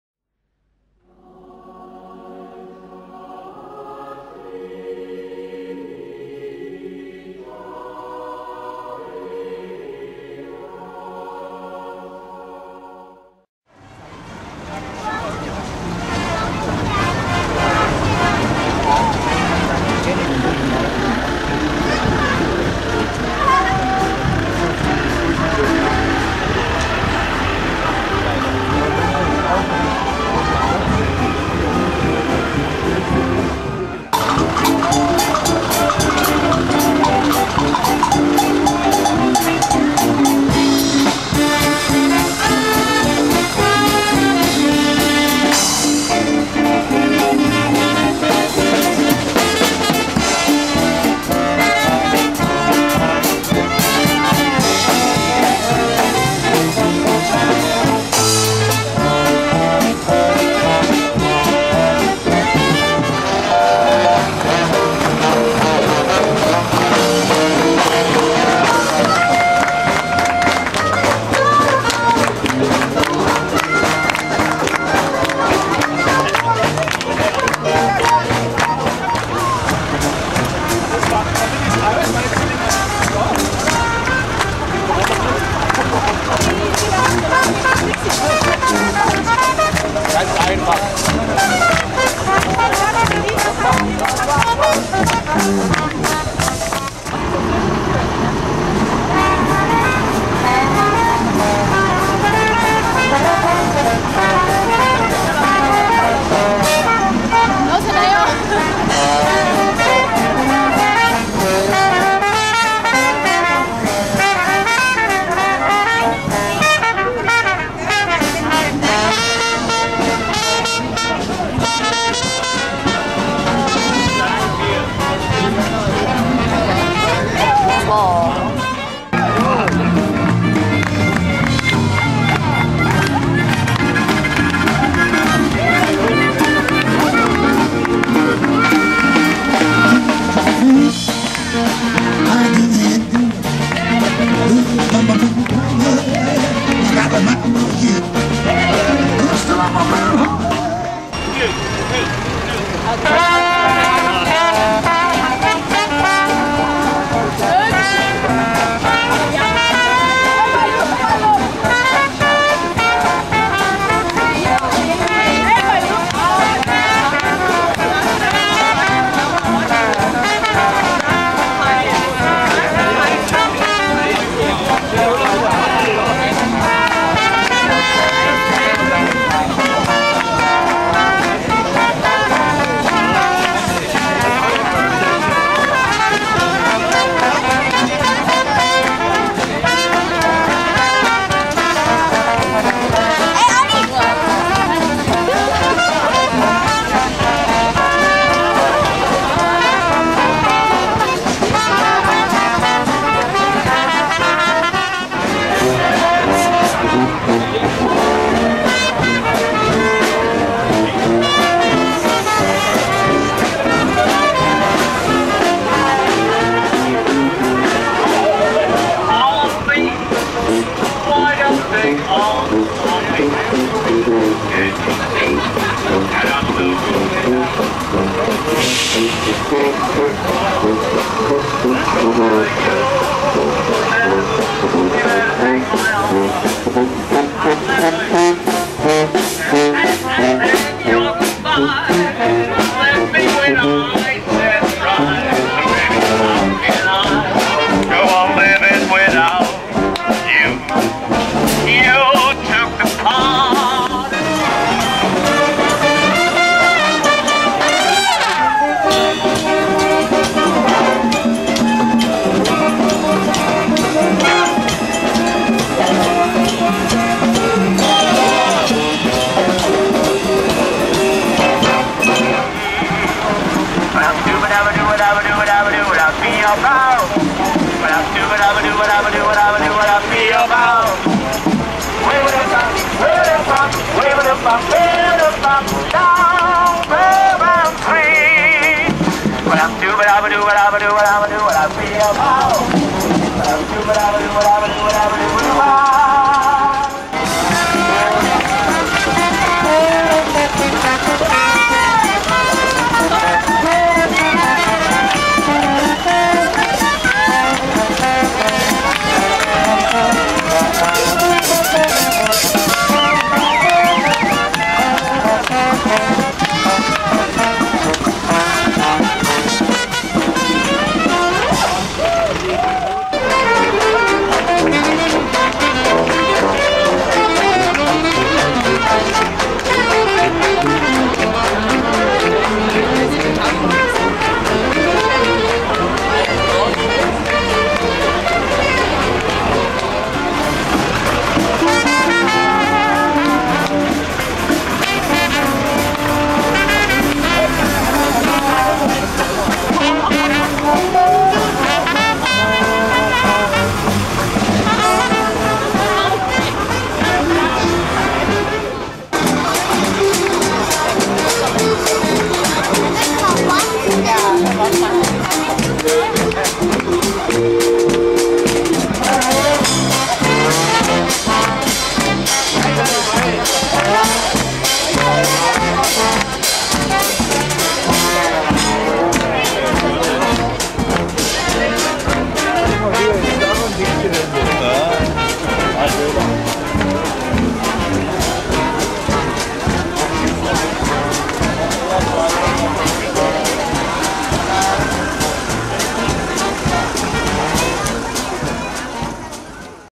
Dresden im Dixiefieber
Ausschnitte aus dem Umzug des Jahres 2002. Beginn des Fahrzeugcorsos in der Haydnstraße in Dresden - Striesen.